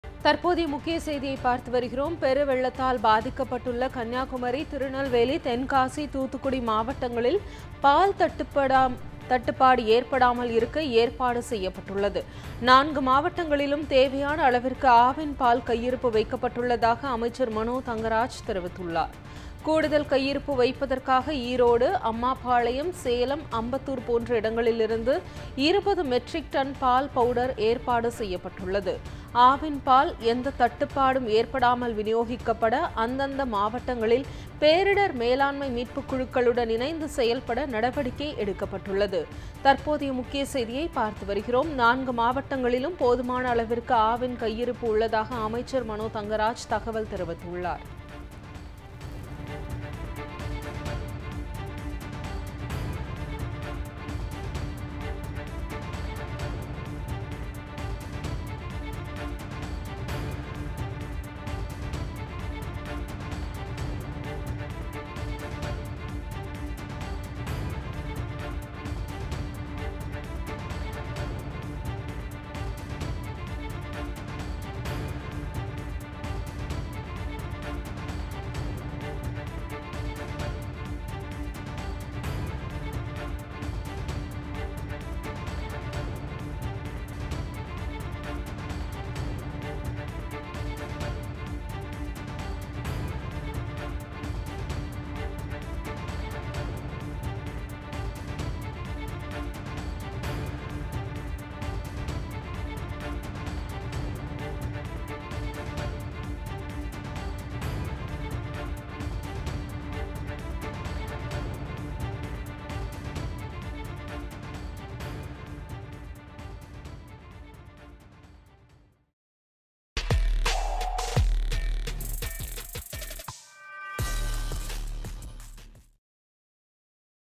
breaking | 4 மாவட்டங்களிலும் போதுமான அளவு ஆவின் கையிருப்பு - அமைச்சர் மனோ தங்கராஜ் தகவல்!
4 மாவட்டங்களிலும் போதுமான அளவு ஆவின் கையிருப்பு - அமைச்சர் மனோ தங்கராஜ் தகவல்!